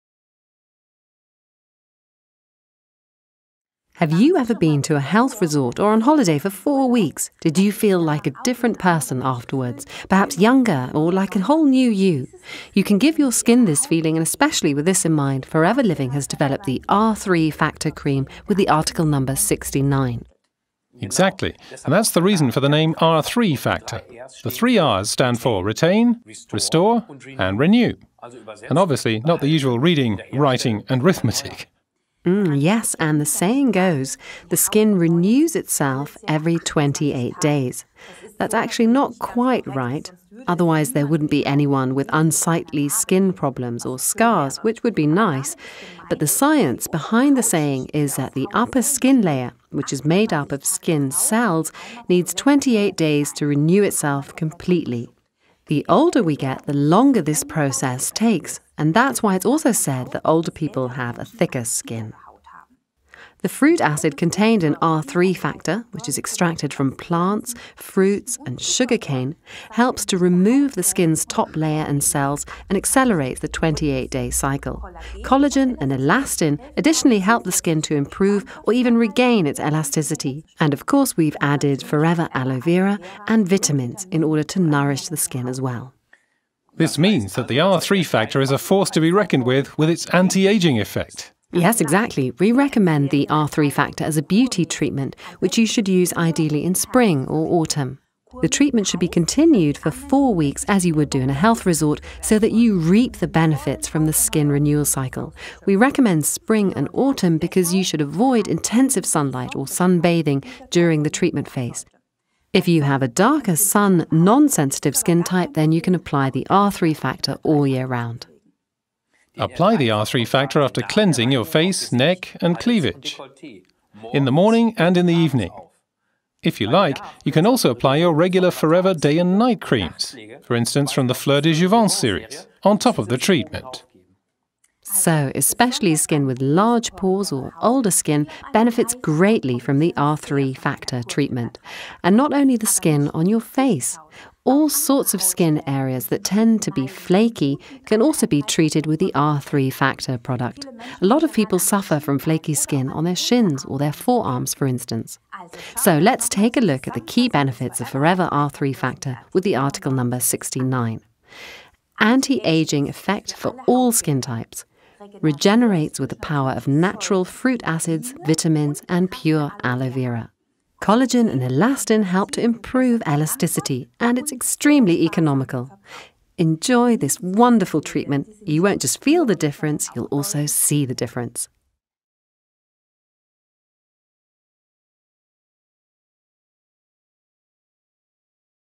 Zaštitna krema sa R³ Factor
R3 faktor je jaka krema koja zaustavlja starenje, smanjuje pojavu bora, vraća prirodnu vlažnost kože, štiti od oštećenja koje izazivaju slobodni radikali, a samim tim pomaže da koža postane glatka i fina.
Preparat sadrži korisne materije kao što su: tokoferol (E vitamin), odnosno acetat koji kao antioksidant sprečava starenje kože i oštećenje ćelijske membrane.
Aloe vera je hidrantna materija koja sadrži emulziju koja pomaže da sastojci i hidratanti prodru u dublje slojeve. Natrijum-hialuronat deluje kao intenzivno hidratantno sredstvo, a linolinska kiselina, koja je masna kiselina, sprečava gubljenje vlažnosti na relaciji ćelija-ćelija.

Sa ovim i sa drugim korisnim materijama koje preparat ima u svom sastavu, R3 Faktor savršeno hrani sve tipove kože i deluje kao hidratantno i kondiciono sredstvo. Primenjuje se takođe u okviru dnevne nege kože.

Način korišćenja: Posle čišćenja kože. nanesite kremu na lice, vrat i druge delove tela gde je potrebno. Za najbolje rezultate koristiti je i ujutru i uveče. Posle nje se može naneti krema za podlogu i šminka ili uveče noćna regenerativna krema, po želji.

- Pomaže očuvanjuzdrave strukture i boje kože
- Zadržava prirodni izgled, obnavlja i podmlađuje vašu kožu

Pakovanje: 57 grama

Aloe Sunscreen

Our Aloe Sunscreen with an SPF of 30, Aloe Sunscreen blocks both UVA and UVB rays. Made with FLP pure, stabilized aloe vera gel, rich moisturizers and humectants, it maintains the skin's natural moisture balance by fighting dryness and helping skin retain moisture.